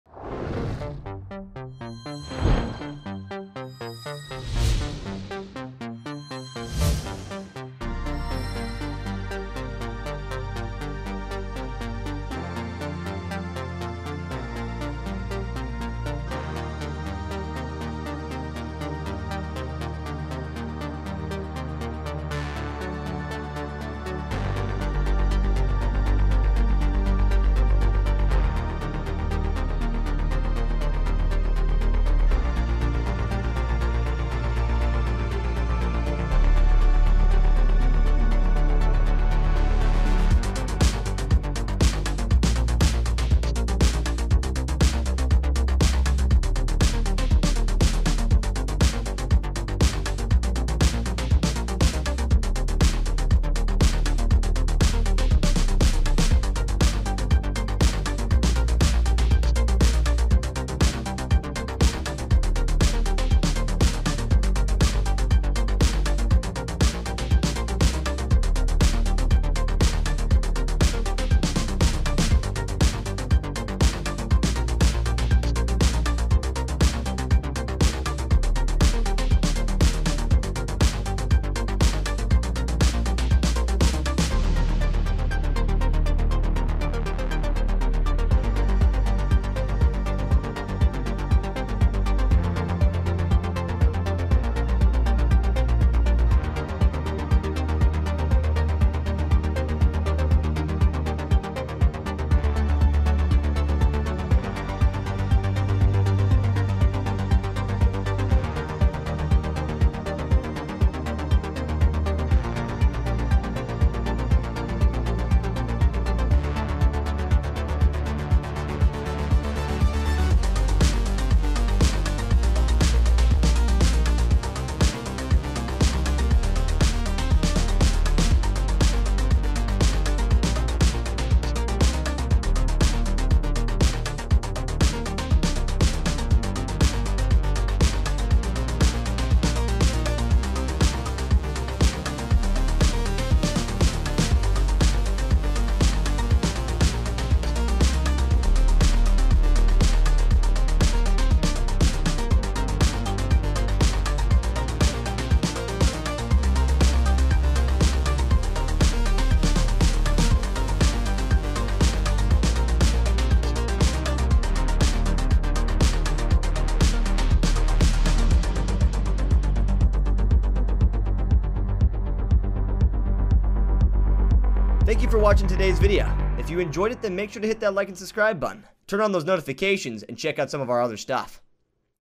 Comparison: Fictional Records
What is the best fictional world record? How many comics has DC's Batman appeared in? How many people has Dragon Ball's Zeno killed? Many of the best Guinness World Records belong to fiction! Like Lord of the Rings' insane kill count and the largest Pokemon collection in history!

Follow our Facebook for Extra content!

Like if you enjoyed this WatchFiction video